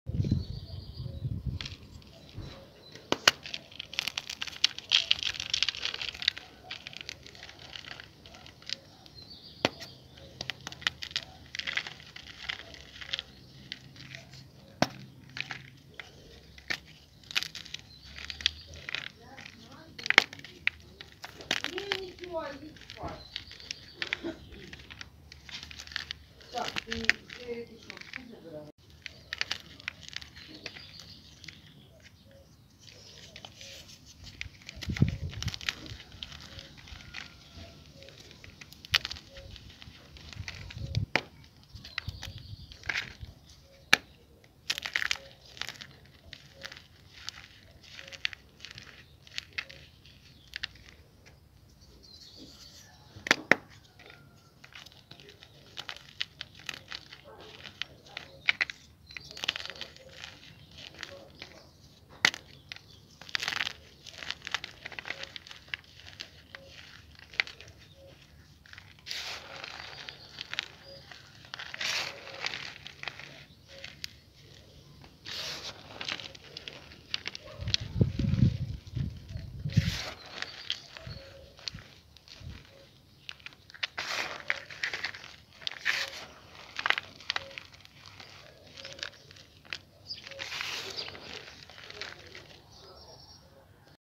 Gym Chalk Block & Babypowder Asmr
I decided to follow this trend too.  So here is my only Gym chalk block I have, dusted with baby powder.  Okay, never mind😂 I just need a broken block for the reforms that I have planned for the June collab)))